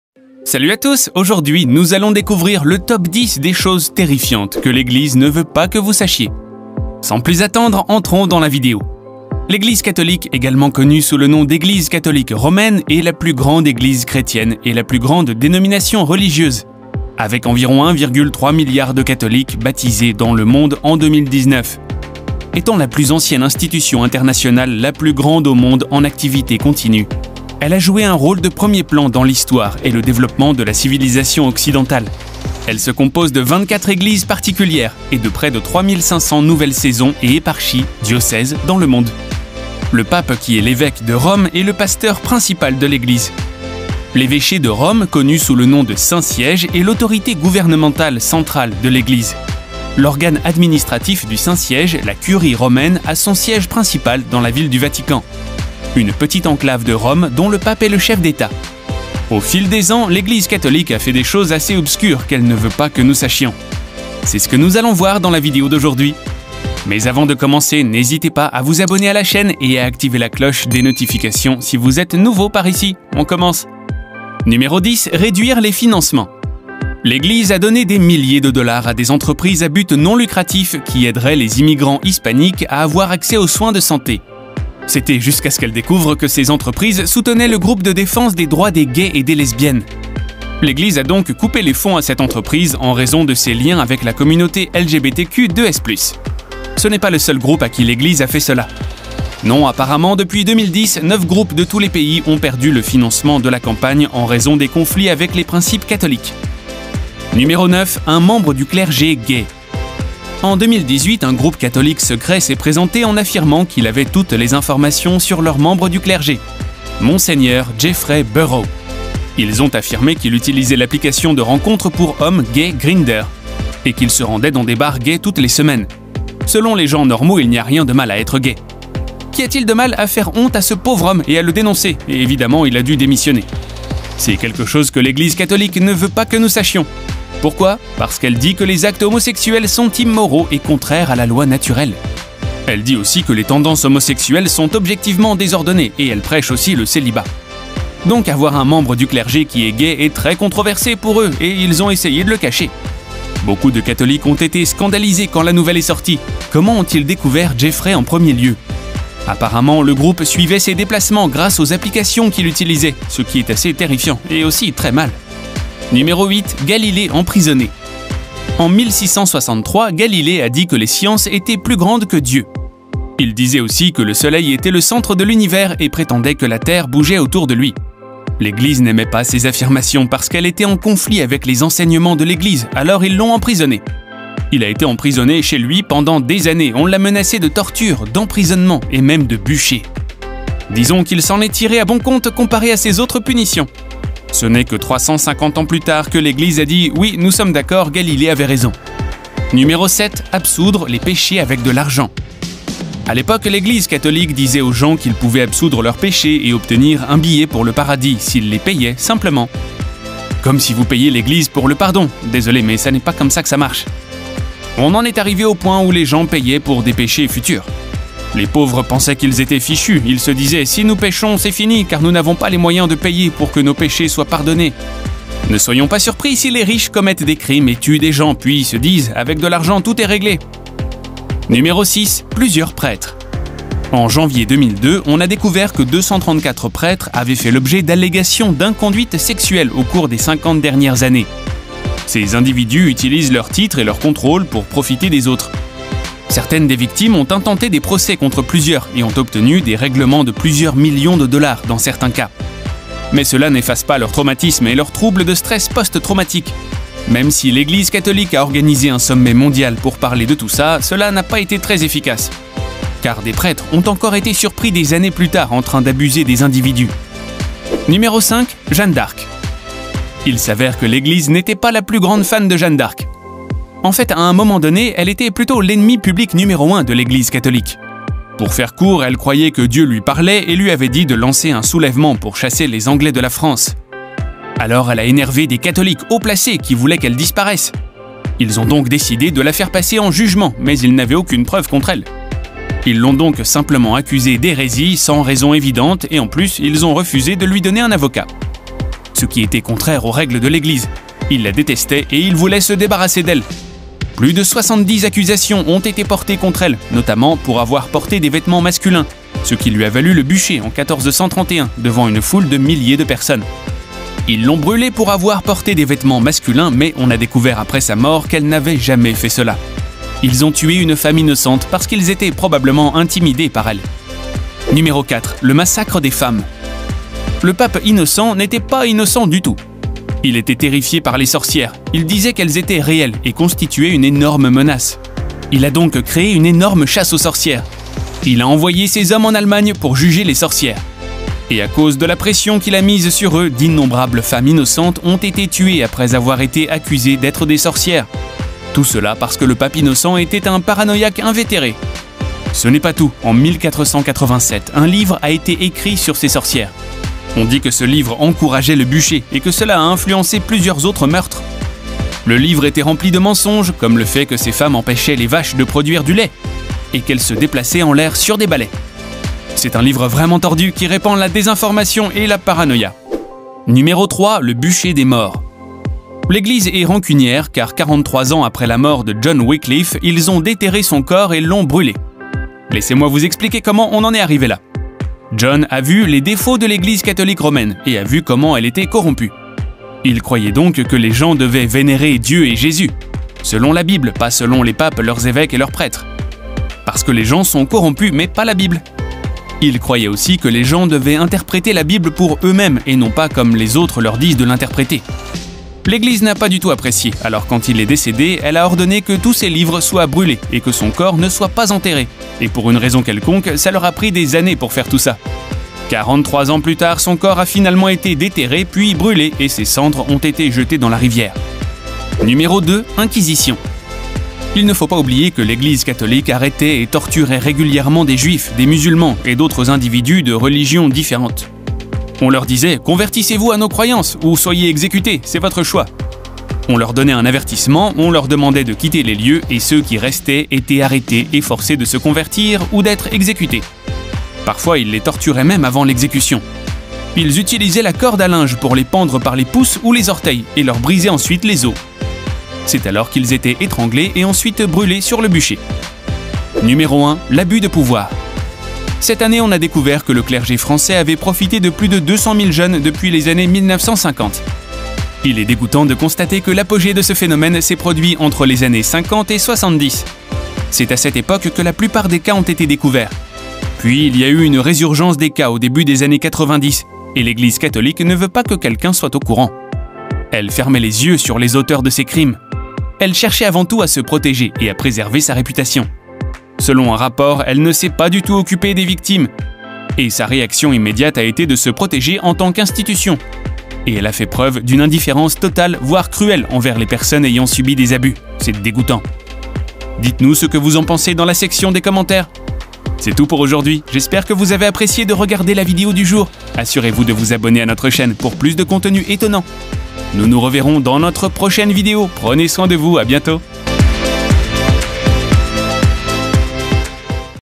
10 SECRETS HORRIBLES QUE LE VATICAN NE VEUT PAS QUE VOUS SACHIEZ | MONDE D'HISTOIRES
10 SECRETS HORRIBLES  QUE LE VATICAN NE VEUT PAS QUE VOUS SACHIEZ | MONDE D'HISTOIRES
Aujourd'hui, nous allons découvrir le top 10 des choses terrifiantes que l'église ne veut pas que vous sachiez. Sans plus attendre, entrons dans la vidéo. L'église catholique, également connue sous le nom d'église catholique romaine, est la plus grande église chrétienne et la plus grande dénomination religieuse avec environ 1,3 milliard de catholiques baptisés dans le monde en 2019.